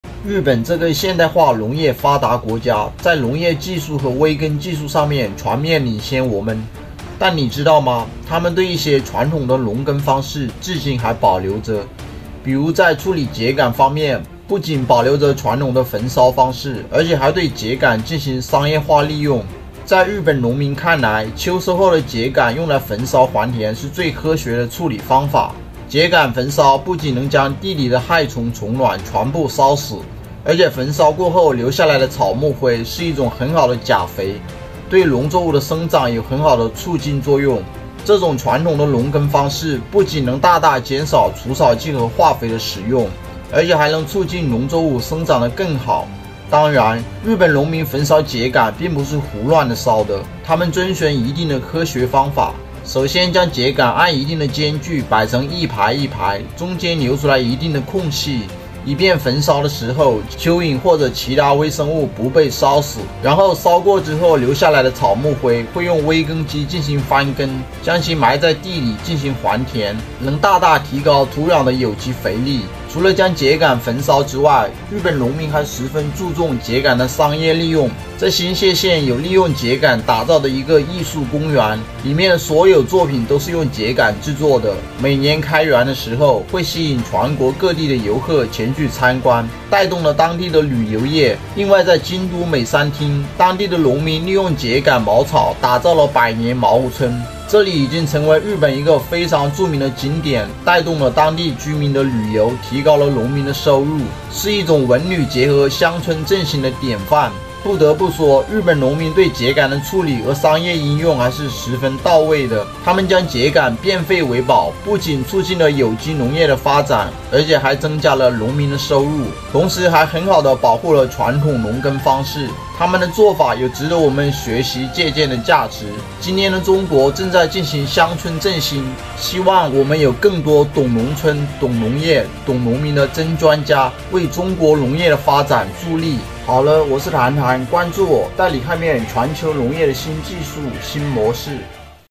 日本至今保留焚燒秸稈的傳統，你知道為什麼嗎？背後的商業邏輯不簡單！
日本這個現代化農業發達國家，在農業技術和微耕技術上面全面領先我們，但你知道嗎？他們對一些傳統的農耕方式至今還保留著，比如在處理秸稈方面，不僅保留著傳統的焚燒方式，而且還對秸稈進行商業化利用。